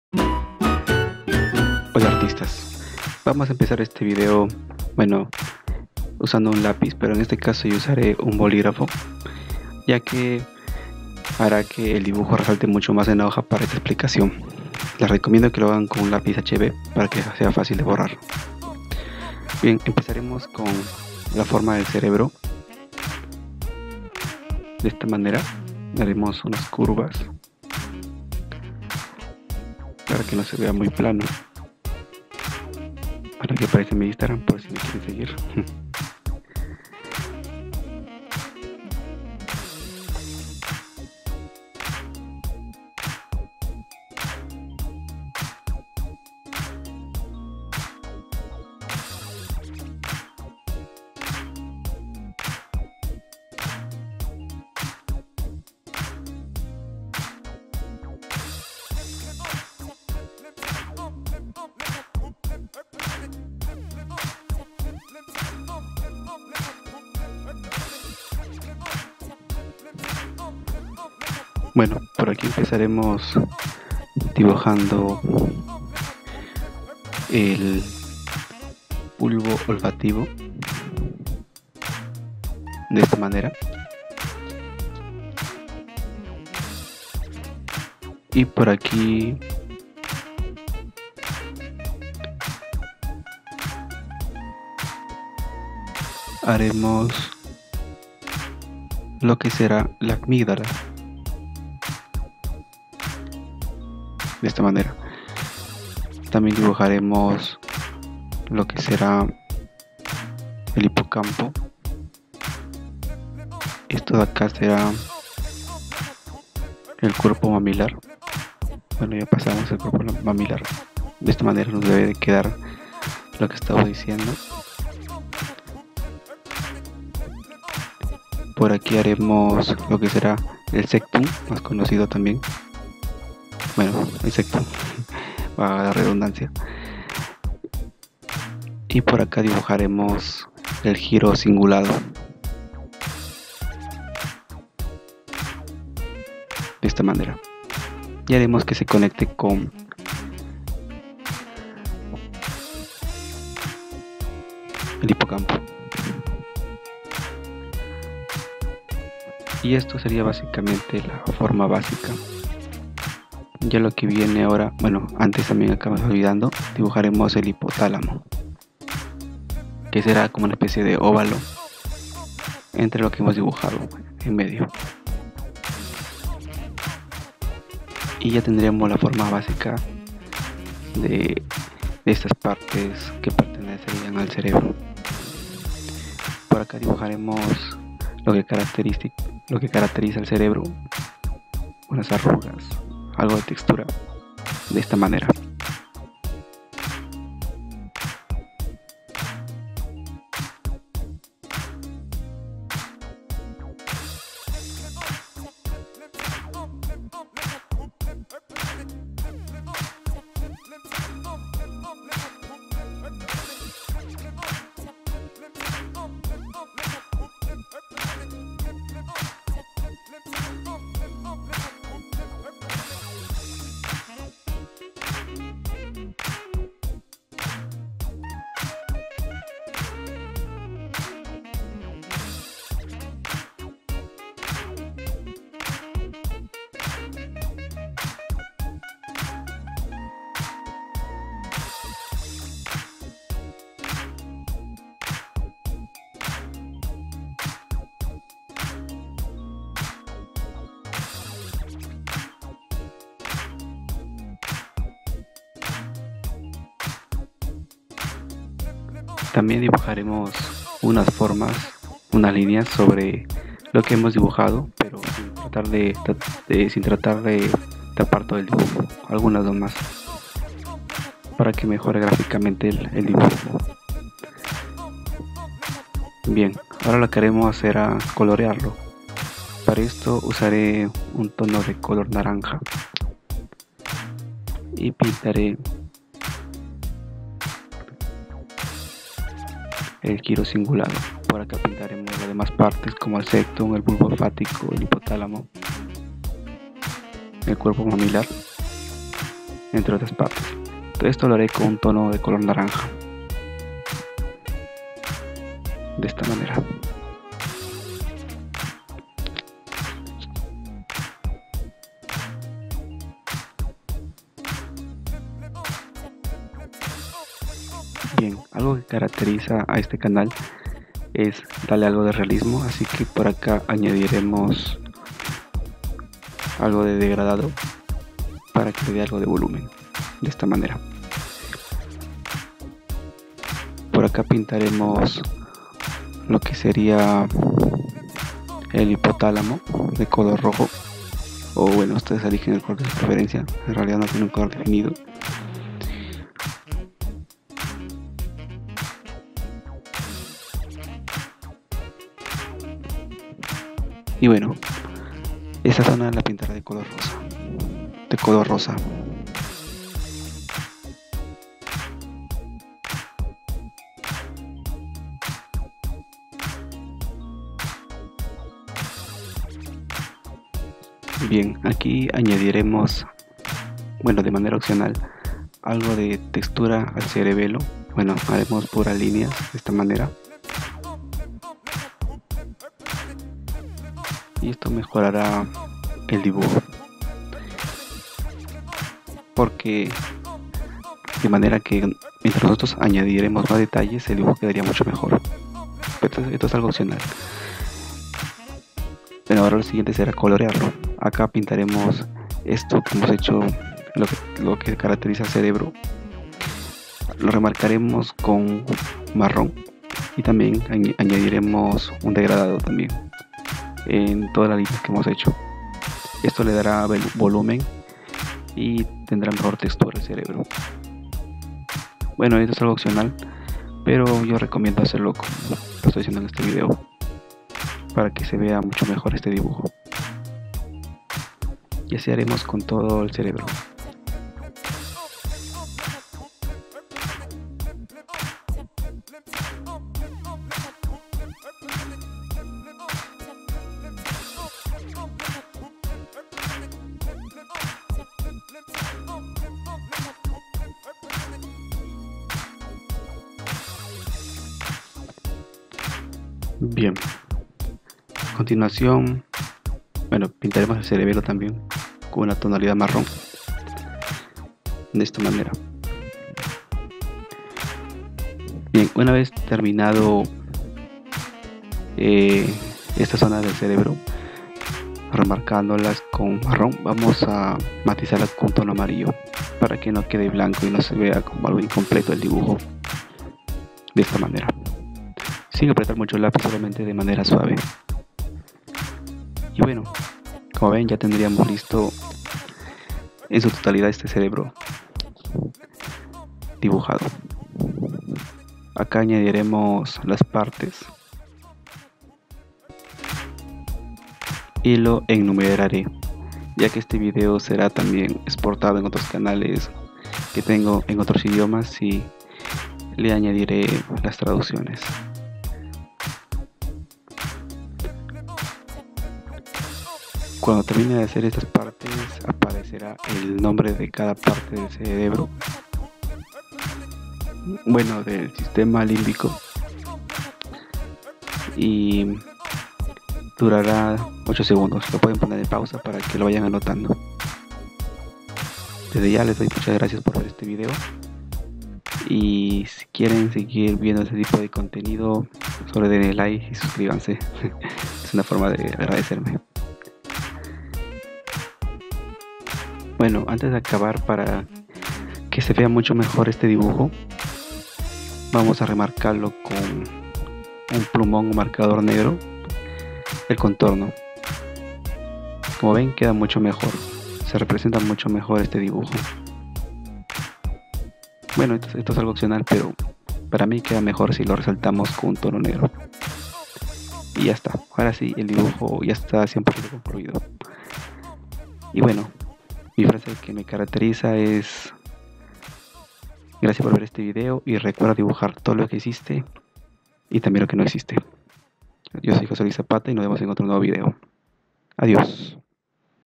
Cómo dibujar el Sistema límbico y sus partes
El sistema límbico es un sistema filogenéticamente antiguo formado por varias estructuras cerebrales complejas que se ubican alrededor del tálamo y por debajo de la corteza cerebral. Es el principal responsable de la vida afectiva.​ En la actualidad se sabe que el sistema límbico está involucrado, junto con otras estructuras más allá de sus límites, en la formación de la memoria, el control de las emociones, las motivaciones, diversos aspectos de la conducta, la iniciativa, la supervivencia del individuo y el aprendizaje.​ Desde el punto de vista funcional, integra estructuras como la circunvolución cingular, la cisura longitudinal, el septo, el cuerpo mamilar del hipotálamo, el fórnix, el hipotálamo, la amígdala cerebral y el hipocampo.

El sistema límbico interacciona muy velozmente (y al parecer sin que necesite mediar estructuras cerebrales superiores) con el sistema endocrino y el sistema nervioso periférico.

-Materiales:
-Lápiz 2B
-Hoja A4
Lápiz color amarillo, naranja y rojo
-Borrador

-Materials:
-Lapiz 2B
-A4 sheet
-Eraser

Cómo dibujar el Cerebro Humano y sus partes